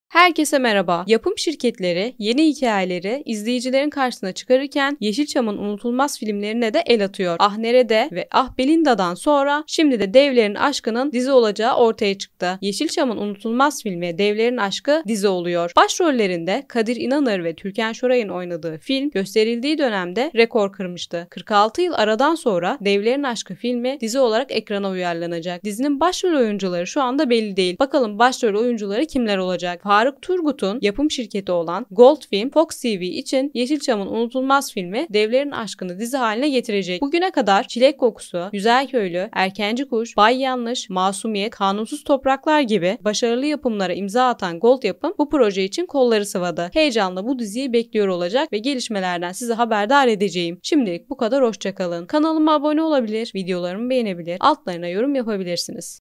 İKİ USTA İSİM YENİDEN |TÜRKAN ŞORAY, KADİR İNANIR, DEVLERİN AŞKI FİLMİ
YEŞİLÇAMIN İKİ USTA İSMİ KADİR İNANIR VE TÜRKAN ŞORAY'IN OYNADIĞI "DEVLERİN AŞKI" FİLMİ DİZİ OLARAK EKRANA UYARLANACAK. DİZİYE DAİR DETAYLARI SİZİNLE BURADAN PAYLAŞACAĞIM. İYİ SEYİRLER.

🟡İzlediğiniz videoları beğendiyseniz "BEĞEN" butonuna basabilir, videolar hakkındaki düşüncelerinizi yorum olarak yazabilirsiniz.